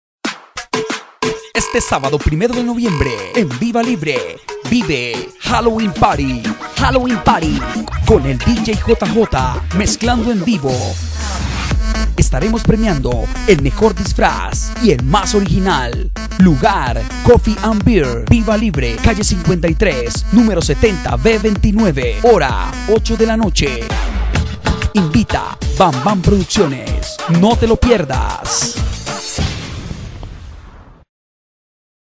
HALLOWEEN PARTY VIVA LIBRE COFFEE AND BEER
Halloween party info
Fiesta de disfraces
Sábado 01 de noviembre en VIVA LIBRE
gran premiacion al mejor y más original disfraz.
Parce
el dj. Es JJ
La dirección es
Calle 53 # 70 d 29
El bar se llama
Coffee and beer Viva libre
Hora 8:00 pm
Invita bam bam production